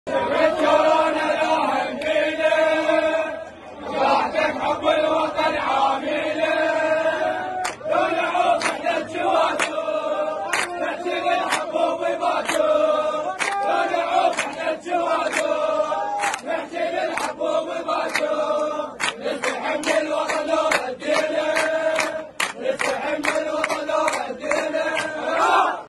متظاهرو قضاء سوق الشيوخ في ذي قار يؤكدون ثباتهم في ساحات التظاهر حتى إسقاط الأحزاب الفاسدة
"لا شفت كورونا لاهم بينه ...وداعتك حب الوطن عامينه"